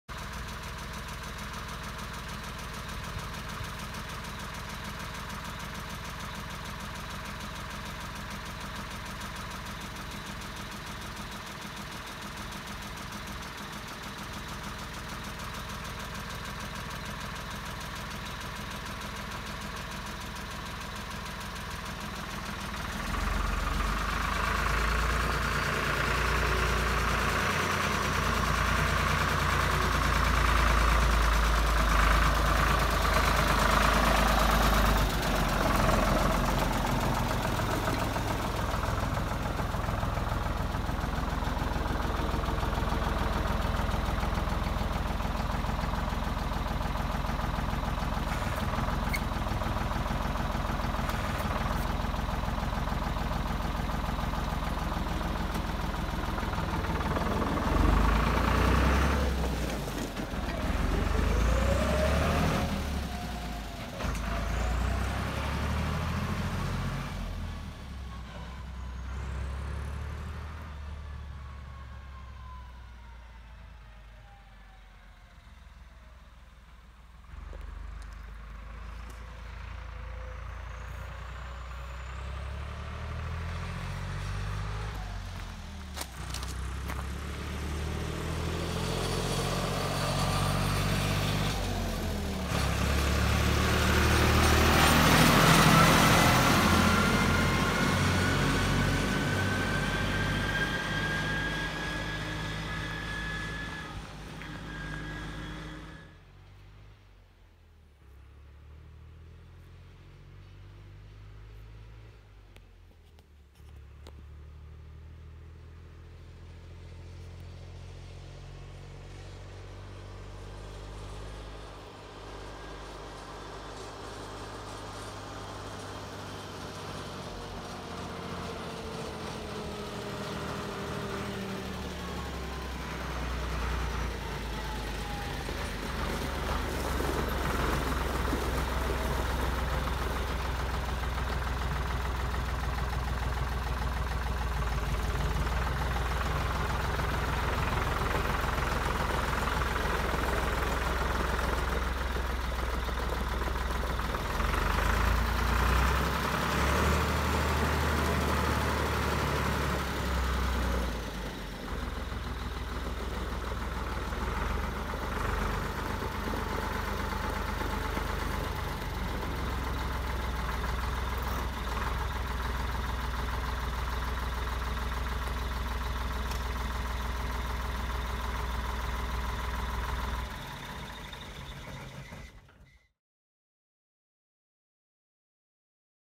Köp Traktor BM Volvo-Valmet 605 4wd + frontlyft på Klaravik.se
Traktor BM Volvo-Valmet 605 4wd + frontlyft
Märke: BM Volvo-Valmet
Modell: 605
Årsmodell: 1984
Driftstimmar: 10751 timmar
Chassinummer: 503181